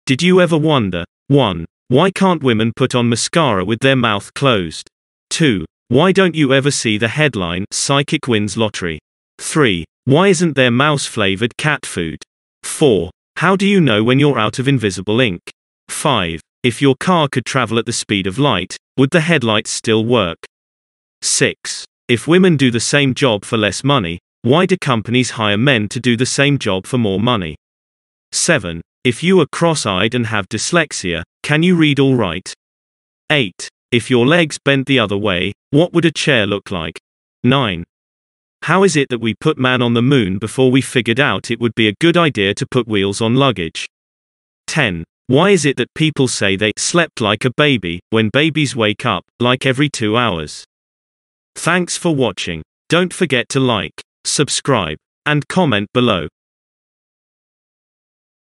5 FUNNY CRAZY THOUGHTS EVER -1
this is a short compilation of jokes and something to think about.
pleases do not take offence to anything in the video as it for comedic purposes.